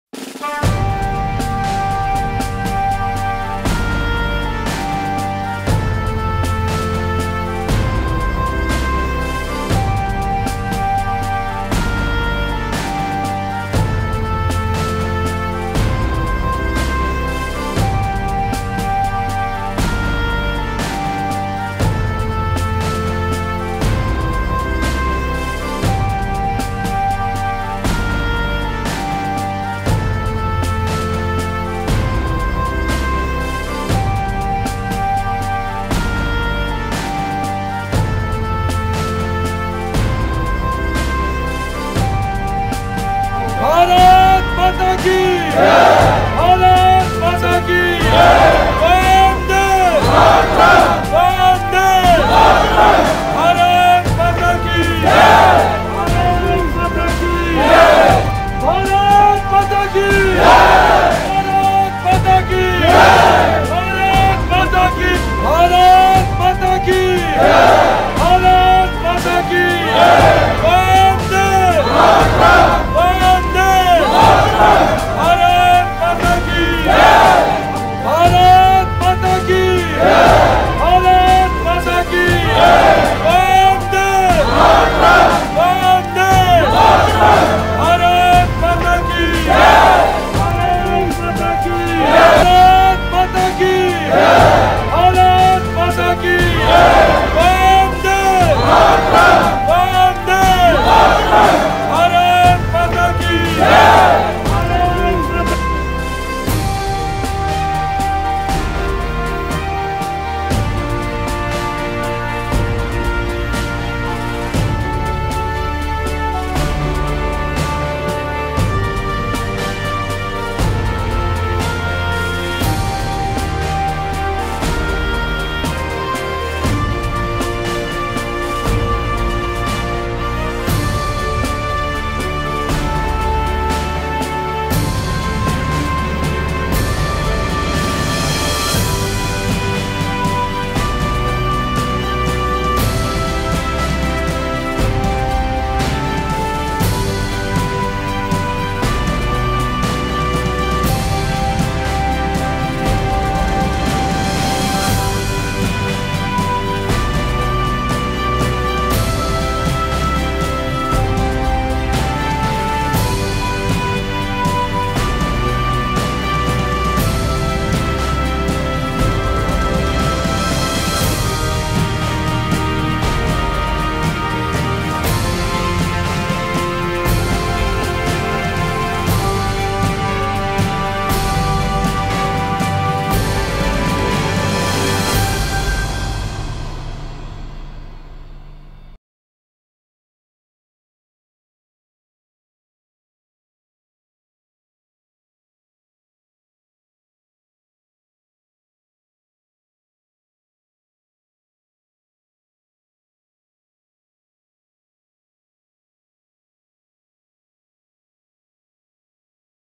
खाई में गिरी सेना की गाड़ी, राजस्थान के लाल भागचंद जी गुर्जर हुए शहीद ~ Rajasthan News
Edited By:  Lokesh
Reported By :
Cameraman :